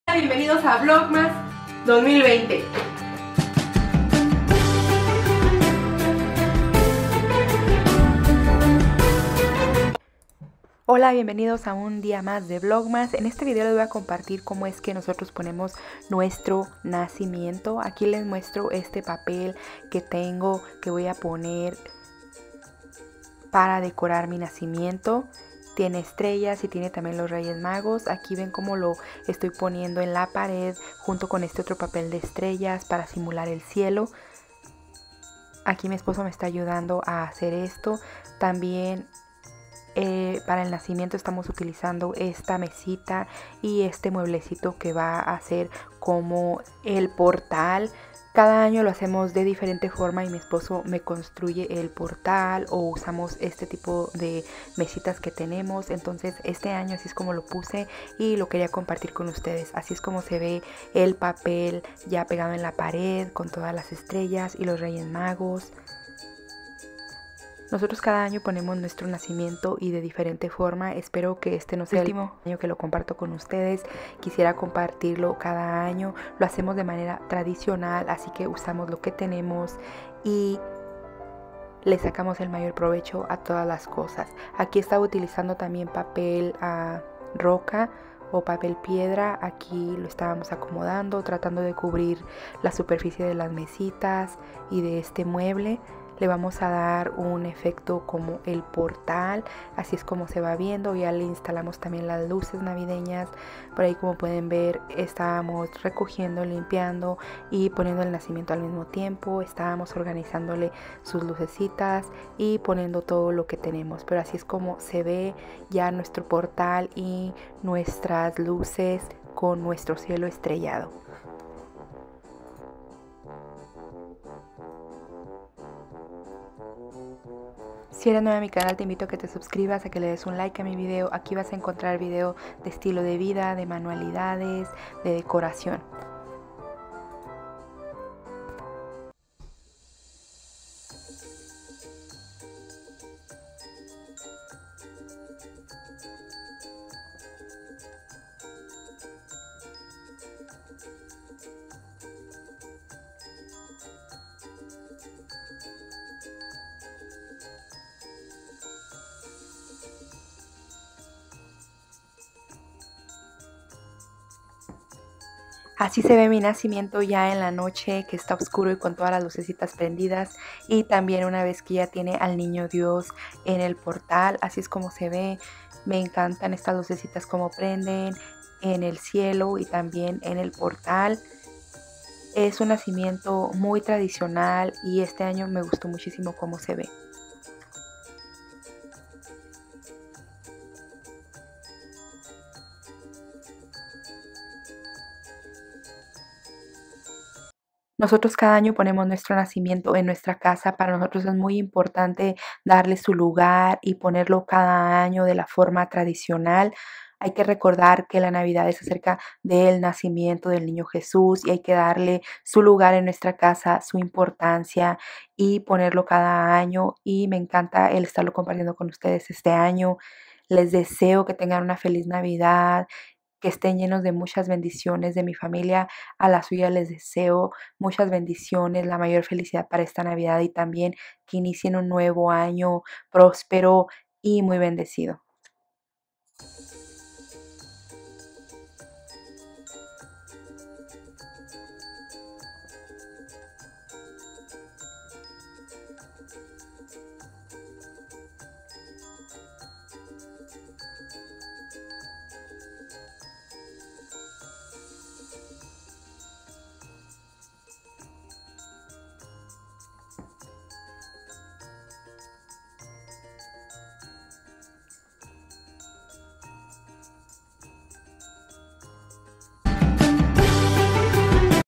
VLOGMAS poniendo el nacimiento
Otro vlogmas mas y te comparto poniendo el nacimiento 2020...